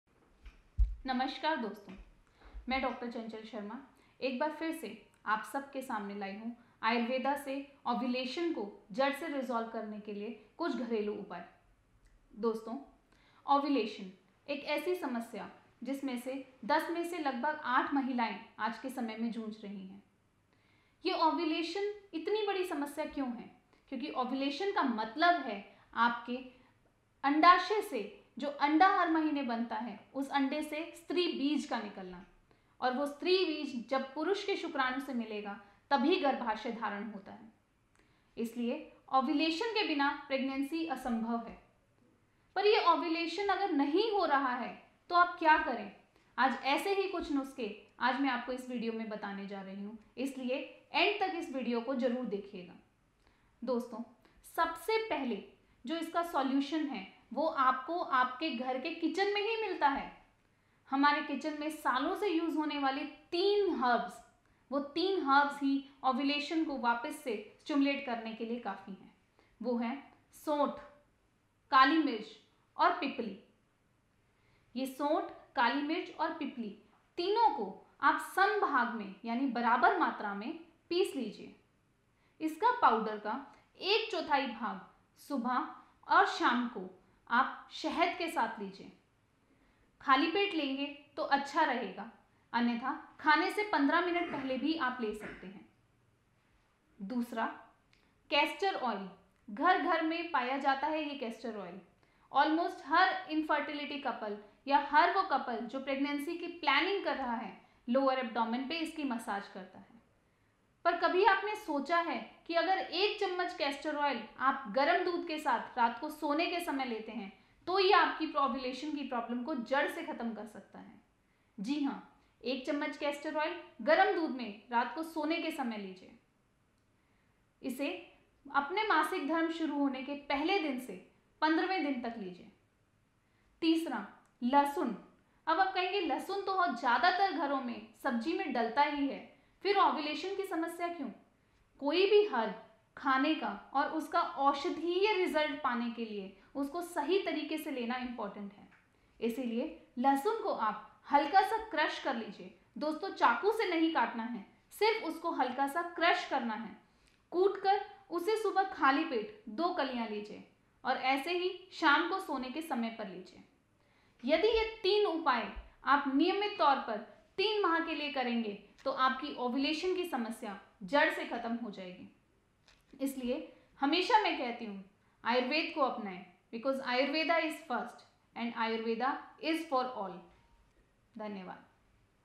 How to improve ovulation | Egg quality बढ़ाने के लिए अचूक घरेलू उपाए
आज हम अपने इस विडियों में बात करेंगे  how to improve ovulation, Egg quality and Boost Fertility by home Remedies |

डॉ चंचल शर्मा जी से जानें.. ovulation को आयुर्वेदा की प्राचीन पद्धति के द्वारा ठीक करने के घरेलु उपाय.. क्योंकि ovulation महिलाओं में एक ऐसी समस्या बन गई है जिससे 10 में से 8 महिलाओं को ovulation से संबंधित दिक्कते होती है। यह ovulation इतनी बड़ी समस्या क्यों है और कैसे इसको आयुर्वेदिक मेडिसिन के द्वारा पूरी तरह से बिना किसी साइड इफैक्ट के दूर किया जाता है और इसकी पूरी विधि इस विडियों में बताई गई है..

call Doctor any query related infertility :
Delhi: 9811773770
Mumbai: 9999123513

What is ovulation day and how to know ovulation day in Hindi?